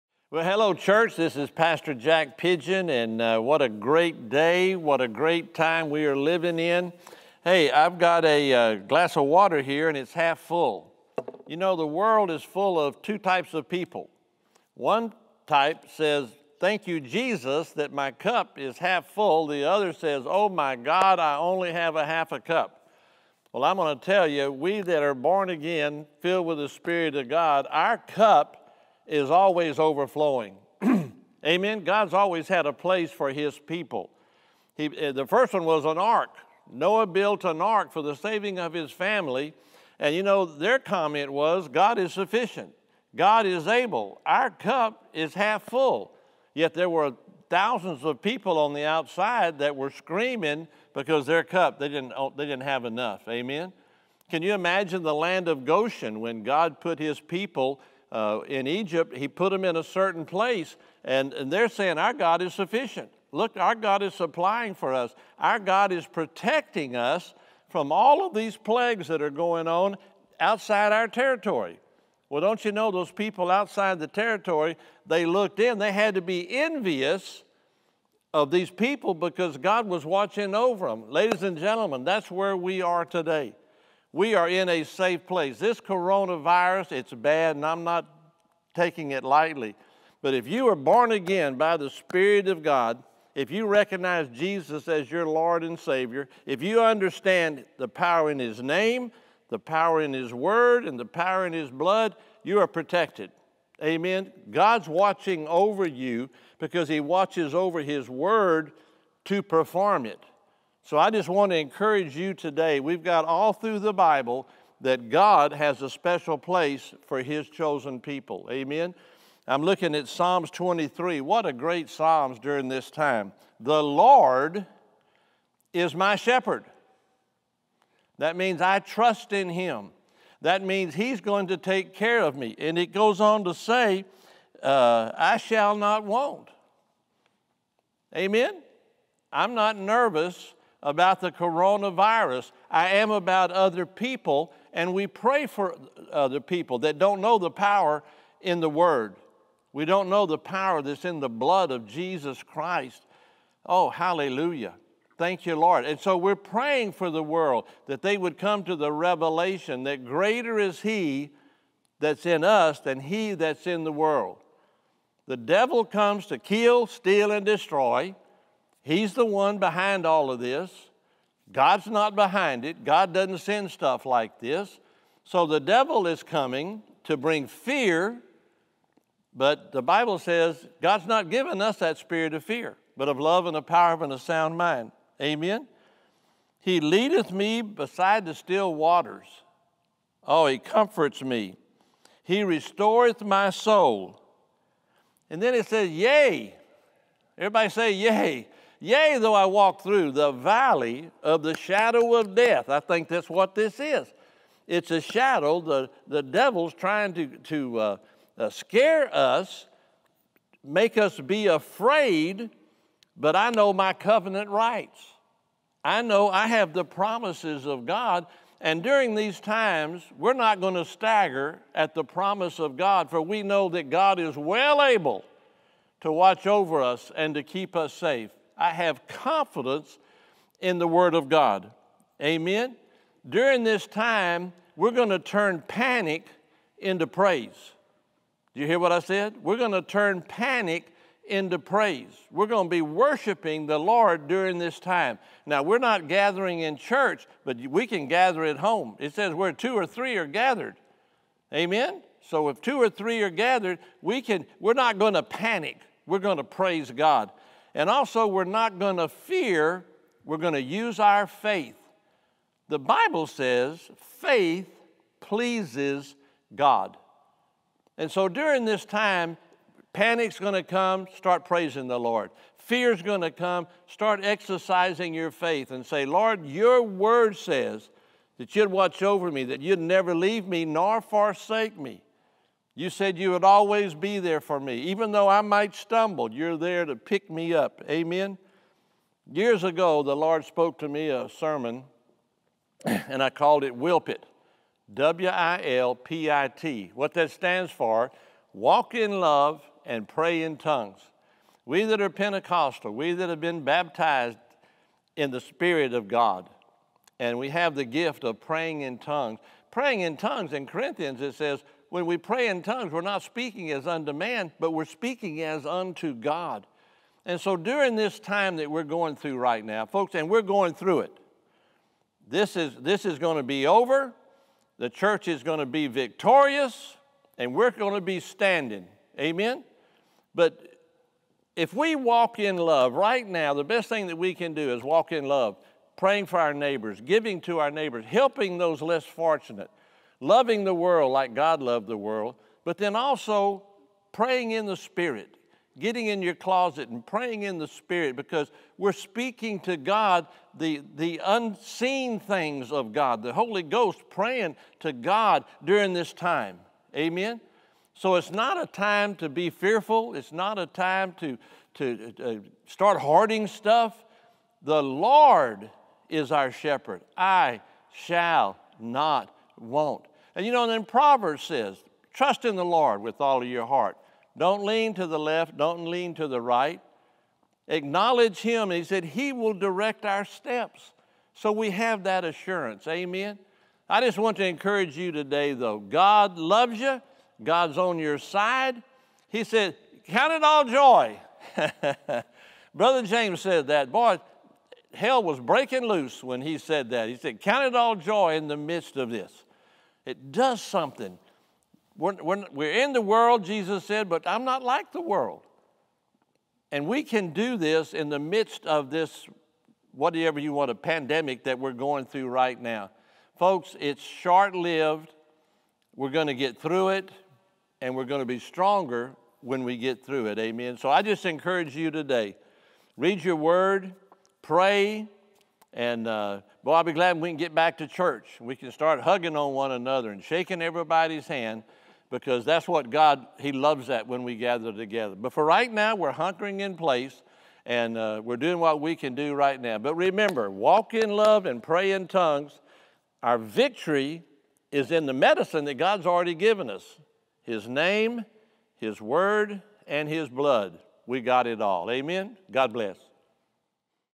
turn panic into praise
This is not a time to panic. God is still on the throne. Take time to listen to an encouraging word from Pastor Jack and make a decision to resist fear and stay in faith.